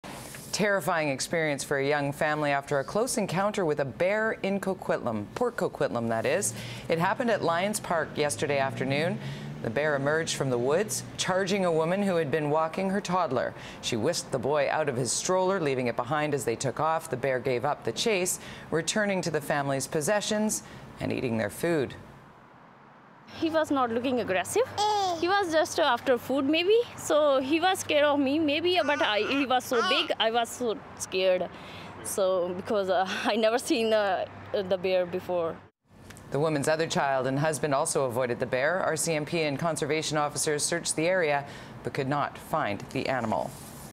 Bear Charges Mother and Toddler
A terrifying experience for a young family, after a close encounter with a bear in Port Coquitlam. A bear charged a woman and her toddler. She whisked the boy out of his stroller, leaving it behind as they took off. The bear gave up the chase.